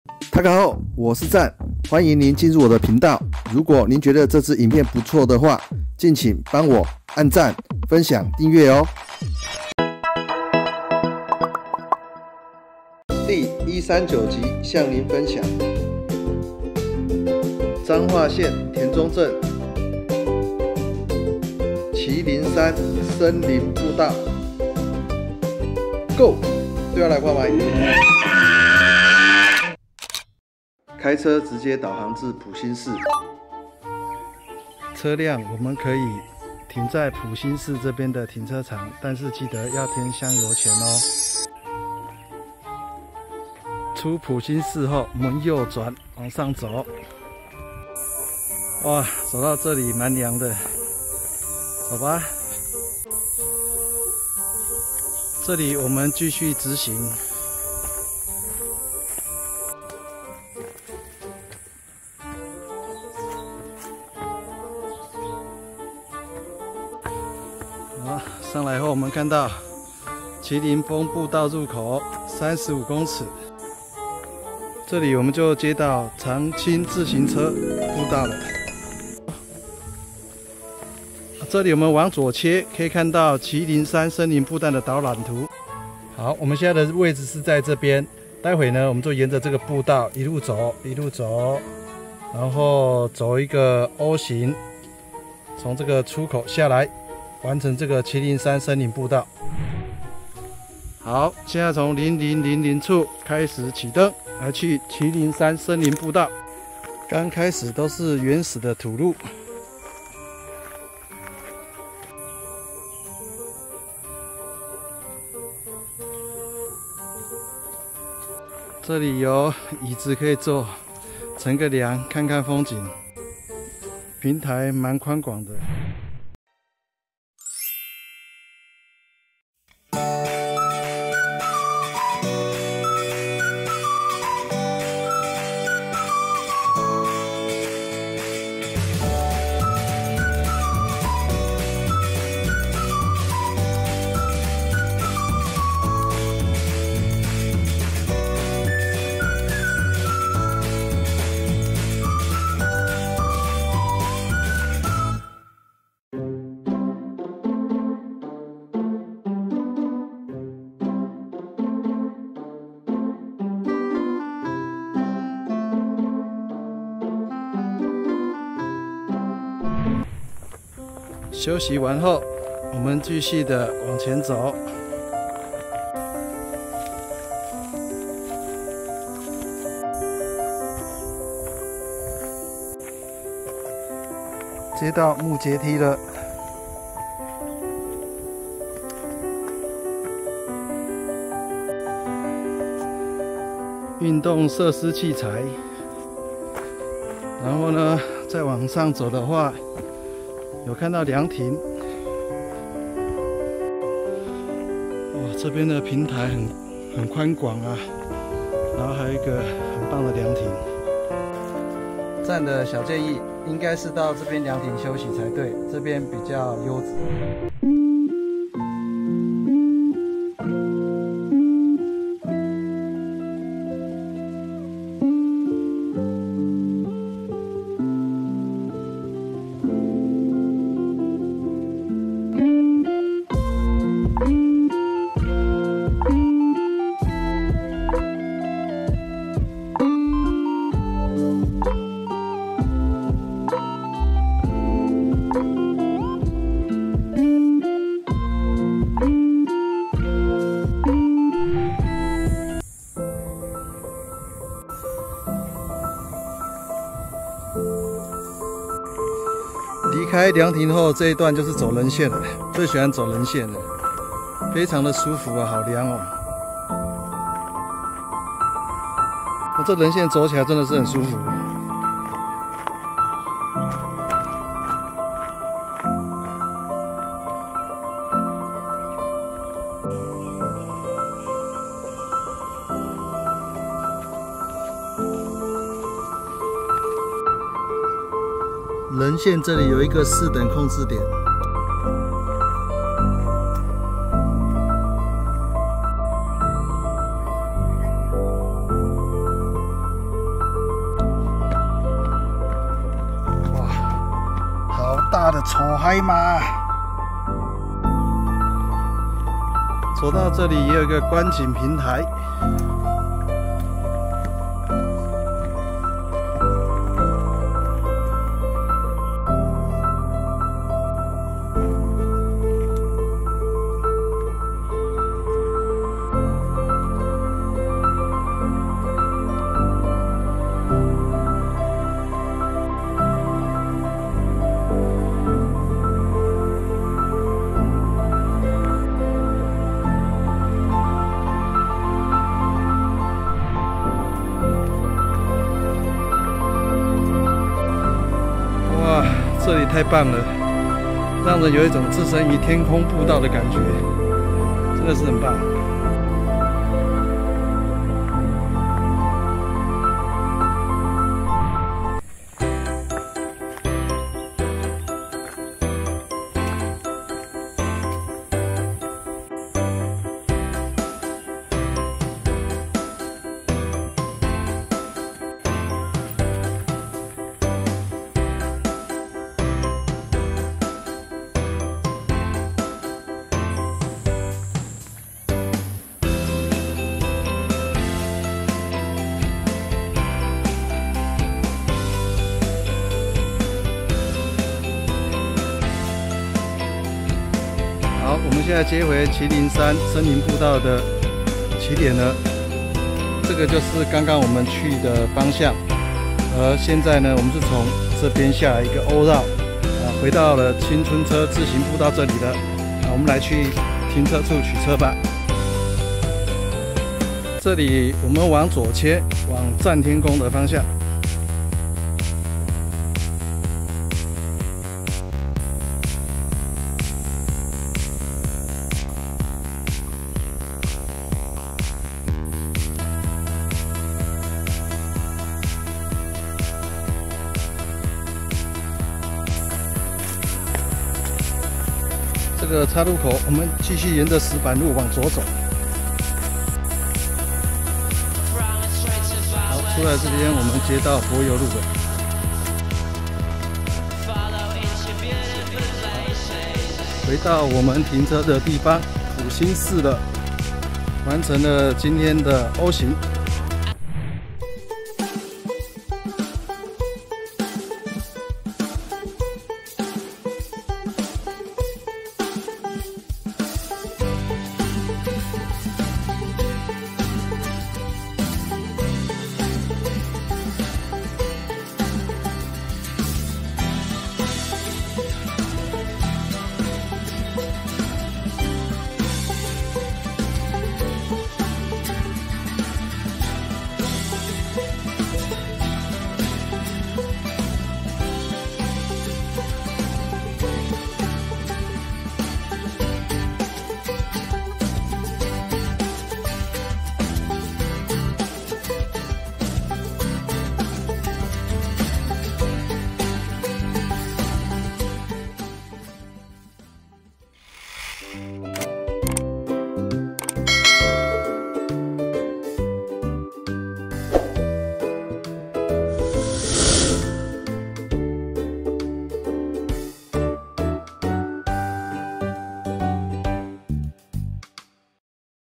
麒麟山森林步道O形（離線地圖在說明欄，歡迎下載）
位於彰化縣田中鎮贊天宮與普興寺的後方，由南投縣林管處採自然工法整建而成，施工時刻意保留原有的大樹，從左右繞道而行，成了饒富趣味與巧思的離樹棧道。

沿途設有多處觀景平台、涼亭和露天體能設施，綠樹成蔭、景致優美，木棧道左鄰深壑、右望山谷，稜線上視野遼闊，能欣賞到南側壯觀的相思樹海和惡地地形的峭壁與深谷，西邊可俯瞰田中市區與彰化平原風光。

全程起伏落差不大，可一邊運動一邊賞景環繞一圈、返回起點，適合全家老小戶外踏青、扶老攜幼同遊山林，下山後可至附近的贊天宮或鼓山寺拜拜，意猶未盡的人可續走香山森林步道，也可選擇騎乘或步行一段旁邊的長青自行車道前往佔地遼闊、設施完善的田中森林公園和赤水崎公園遊玩，同享自然健康的天倫之樂。
登山日期：111.5.20